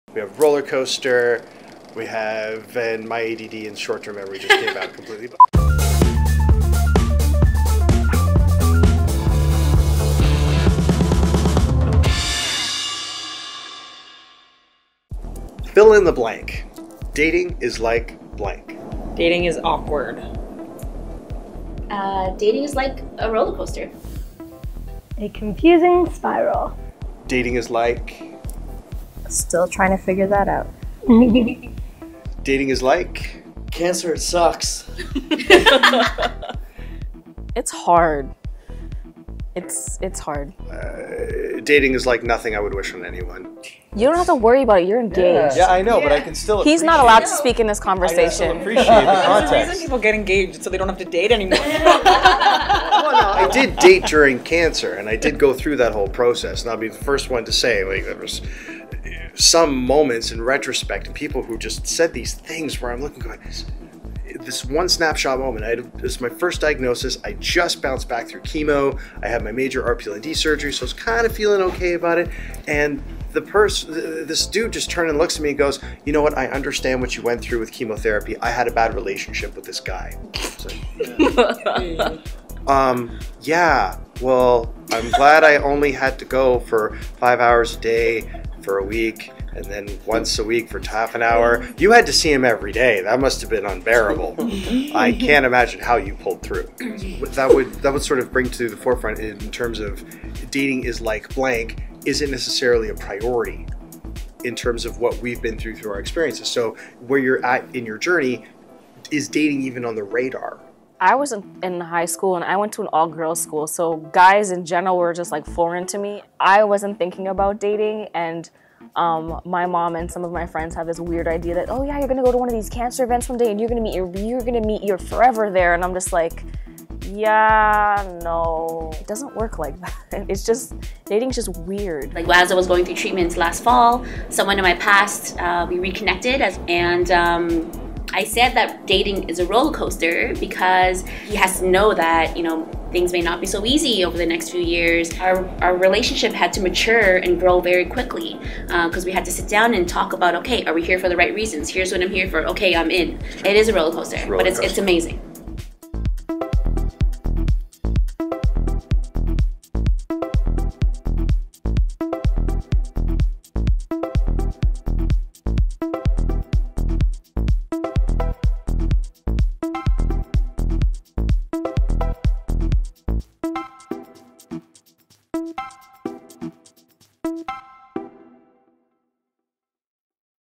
Dating is Like... // The C Word: Young Adults with Cancer
Dating is Like...

A group of young adult cancer survivors candidly answer questions about life, being young, and having cancer.

Panel:
Stephanie Sliekers
Carolyn Van
Kyla Demarco
Vinesha Ramasamy
Thomas Cantley
Alyssa Whiteman
Peter Laneas

Support for Adolescents and Young Adults living with cancer:

There are programs available at Princess Margaret and in the community that support adolescents and young adults living with cancer.